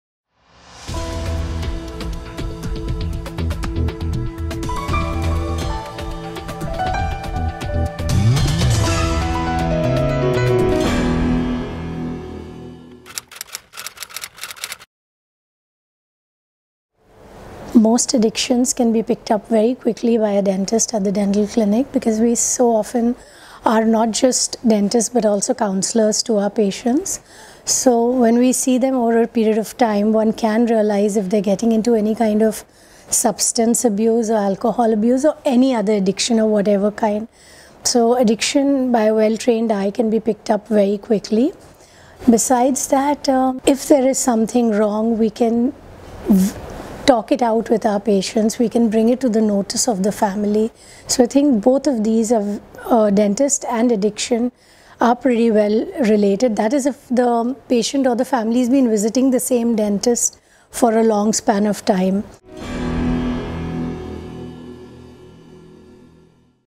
Anatta Speaks Dentistry & Addiction Dr. Neetika Modi
Anatta Speaks, Celebrity Cosmetic Dentist
Relation Between Addiction & Dentistry